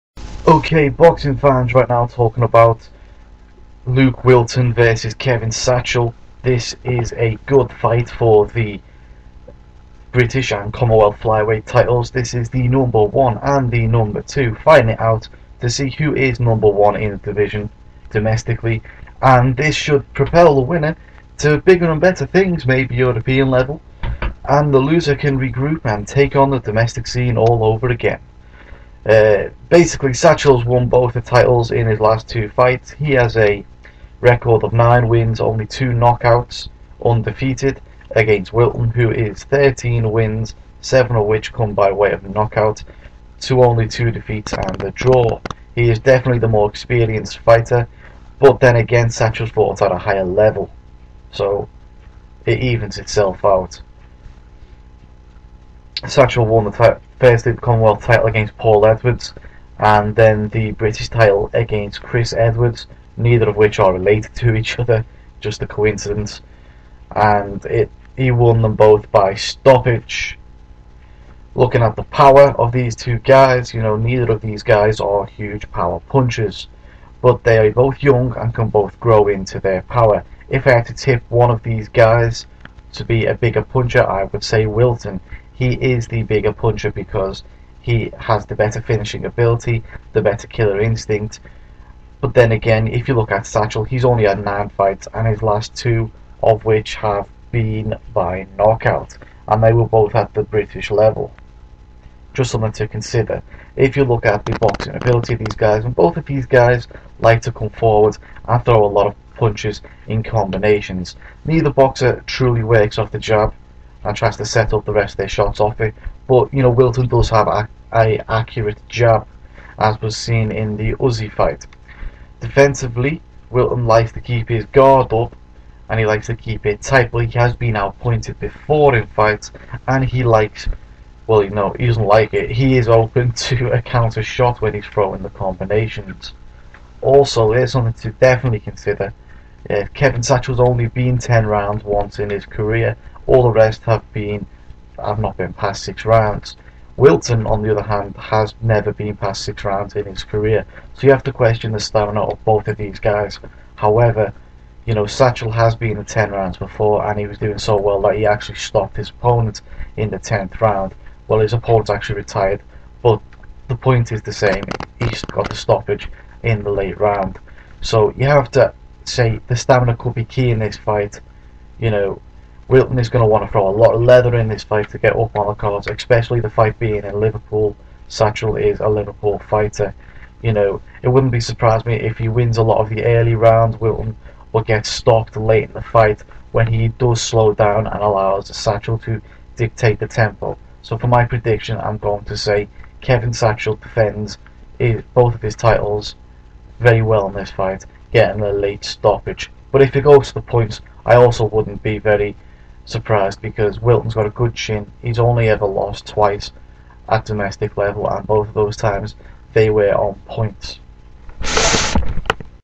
Kevin Satchell Vs Luke Wilton Prediction
first defence of his title by Satchell against a top quality opponent and number 2 in the british rankings